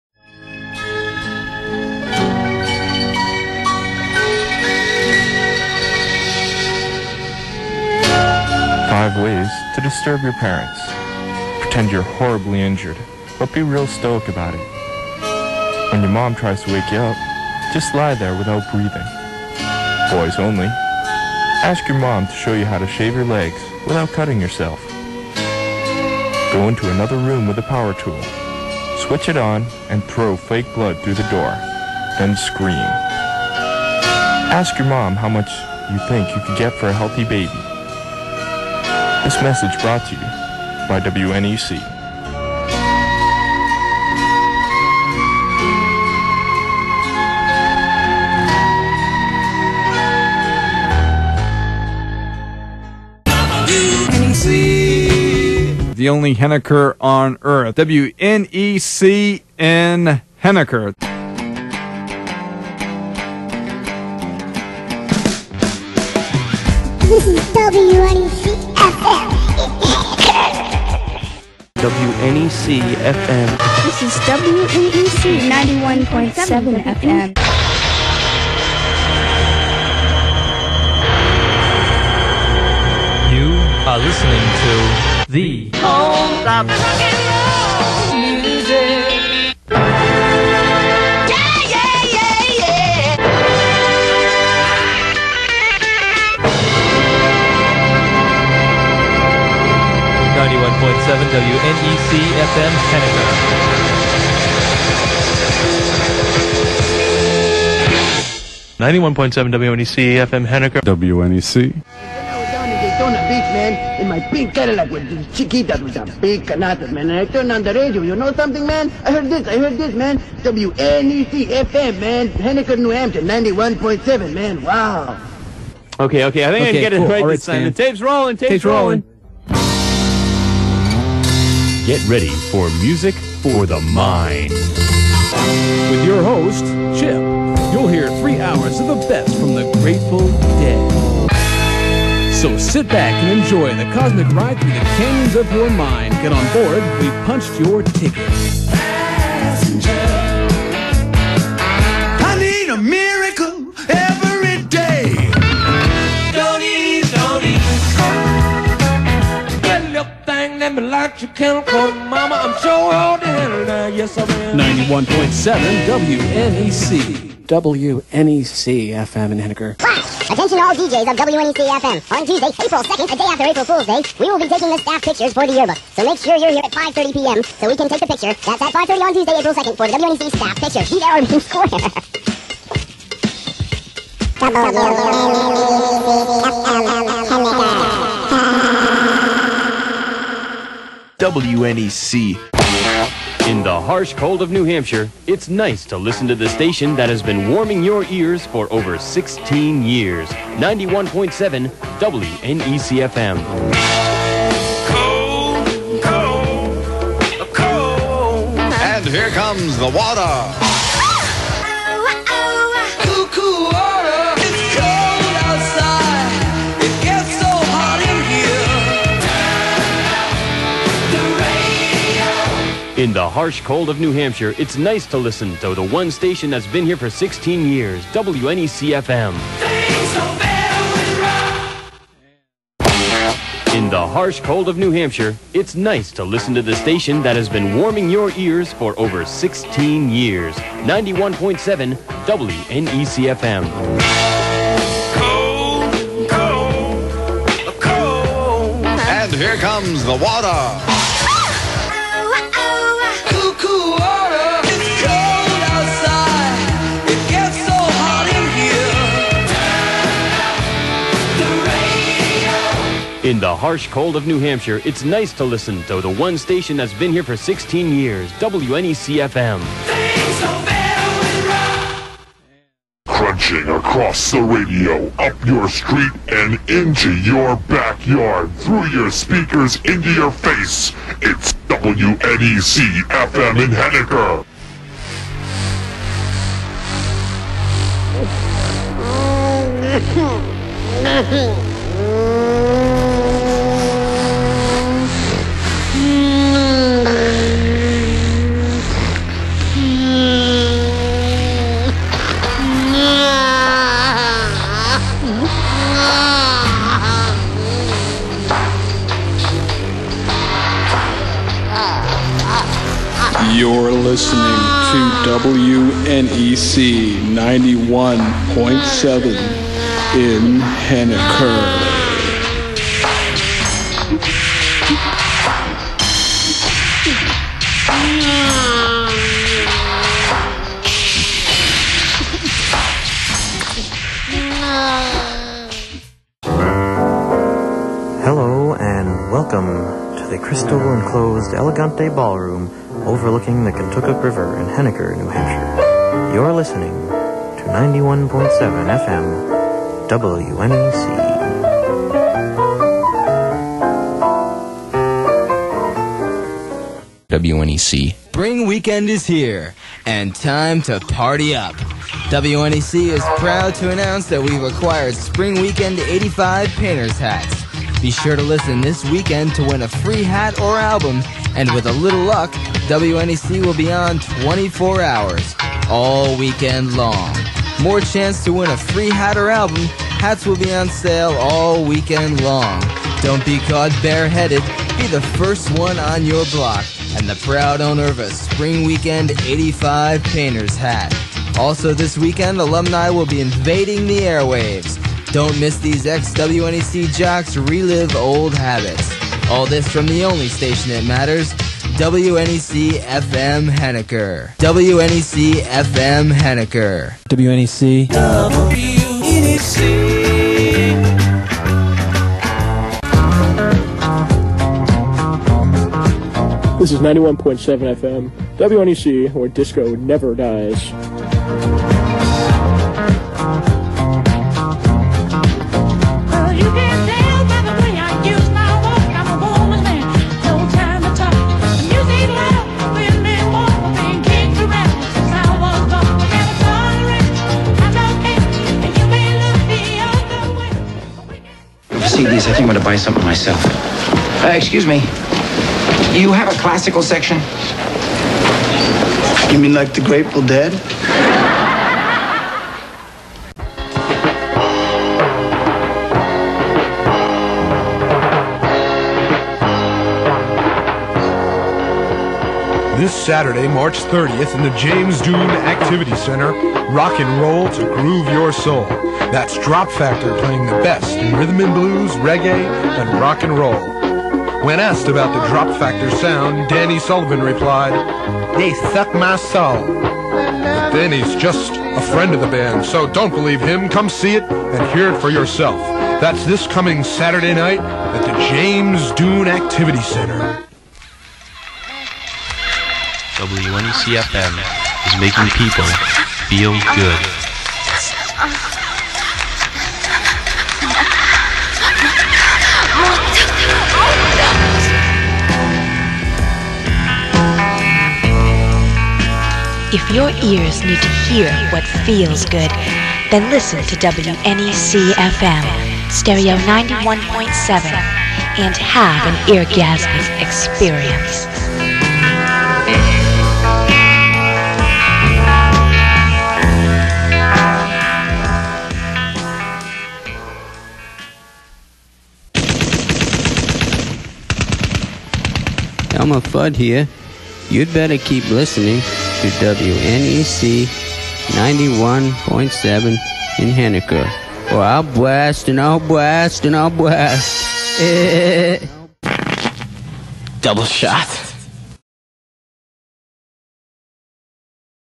WNEC Snippets Vol. 1
A collection of vintage production pieces and random silliness by the DJs of WNEC FM dating back as far as the early 1980s.